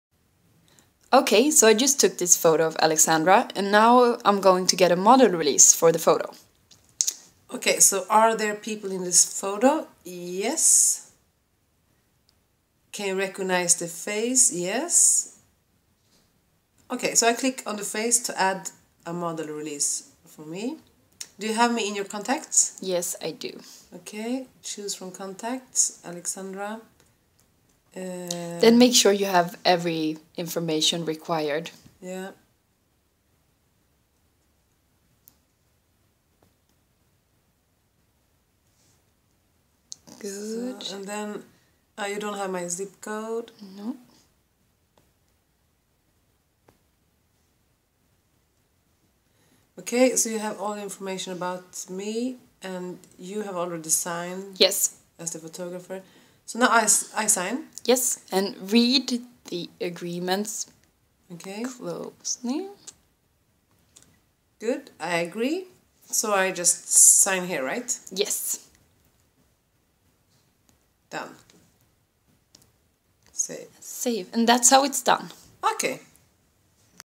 Foap introducing Model release
We've learned that our brands want to be sure that a person in a photo has agreed to be in a photo for commercial sale. Here's a guided tour in how to create a Model release in the Foap app.